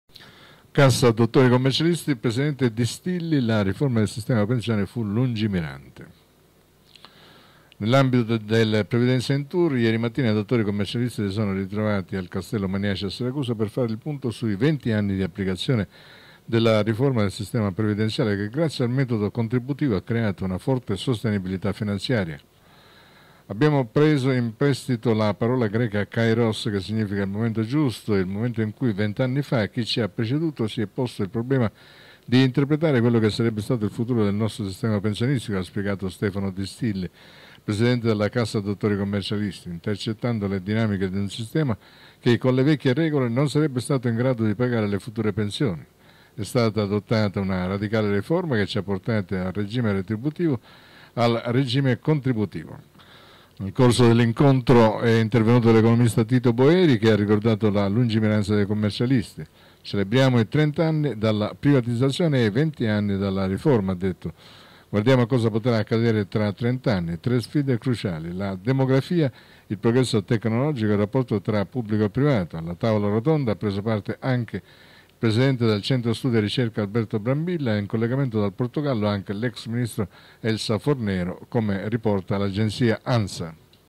Cassa dottori commercialisti, presidente Distilli: «La riforma del sistema pensioni fu lungimirante»
Nell’ambito del “Previdenza in tour” ieri mattina i dottori commercialisti si sono ritrovati al Castello Maniace a Siracusa per fare il punto sui 20 anni di applicazione della riforma del sistema previdenziale che grazie al metodo contributivo ha creato una forte sostenibilità finanziaria. “Abbiamo preso in prestito la parola greca Kairos che significa il momento giusto, il momento in cui vent'anni fa chi ci ha preceduto si è posto il problema di interpretare quello che sarebbe stato il futuro del nostro sistema pensionistico - ha spiegato Stefano Distilli, presidente della Cassa dottori commercialisti -. Intercettando le dinamiche di un sistema che con le vecchie regole non sarebbe stato in grado di pagare le future pensioni. E’ stata adottata una radicale riforma che ci ha portati dal regime retributivo al regime contributivo.
E il sistema ha saputo rendersi sostenibile dal punto di vista finanziario e patrimoniale". "Abbiamo un patrimonio di 11 miliardi di euro - ha continuato il presidente Distilli - che sarà proprio quello che ci permetterà di far fronte all'invecchiamento della nostra platea, di tutti coloro che nei prossimi anni andranno in pensione. Questa solidità patrimoniale ci ha permesso anche di implementare la seconda missione della nostra cassa e di ogni ente previdenziale, quella assistenziale, creando un sistema di welfare che supporta i colleghi nelle fasi di difficoltà, ma soprattutto sempre più mirato a supportare i colleghi nell'avvio della professione, nello sviluppo della professione attraverso gli investimenti, ma soprattutto attraverso la formazione e l'acquisizione di nuove competenze”.
Nel corso dell’incontro moderato è intervenuto l’economista Tito Boeri che ha ricordato la “lungimiranza dei commercialisti. Celebriamo i 30 anni dalla privatizzazione e i 20 anni dalla riforma - ha detto -. Guardiamo a cosa potrà accadere tra 30 anni. Tre sfide cruciali: la demografia, il progresso tecnologico, il rapporto tra pubblico e privato”. Alla tavola rotonda ha preso parte, anche il presidente del Centro studi e ricerche Alberto Brambilla e, in collegamento dal Portogallo, anche l’ex ministro Elsa Fornero, come riporta l’agenzia Ansa.